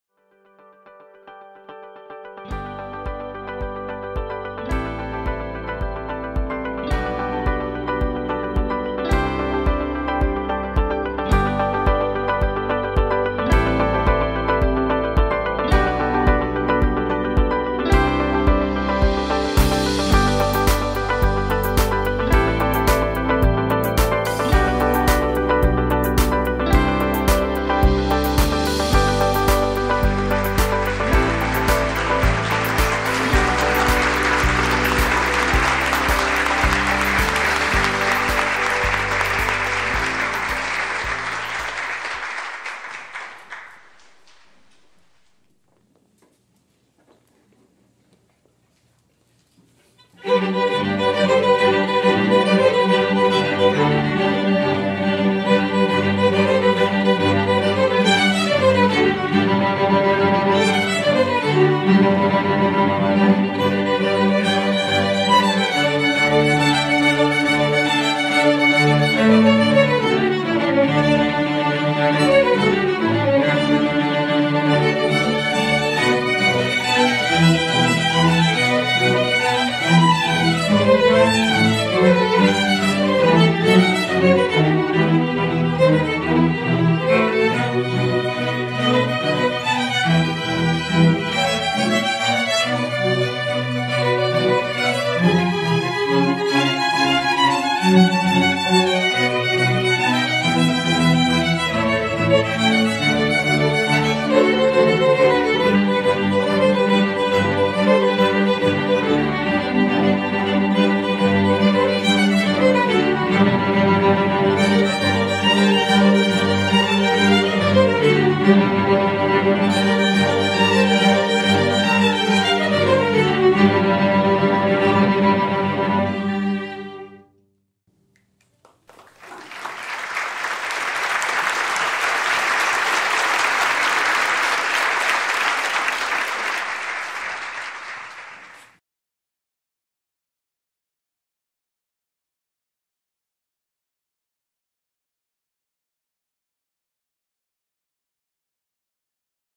Concierto en G Mayor - Casa de Retiro - La costa Glen
Jovenes de la Orquesta Mainly Mozart de San Diego Ca. en apoyo a adultos de la 3ra edad, en una casa de retiro, comparten su talento brindando emociones y un ratito de esparcimiento.

La experiencia fue mas que grata en un lugar digno de un evento asi.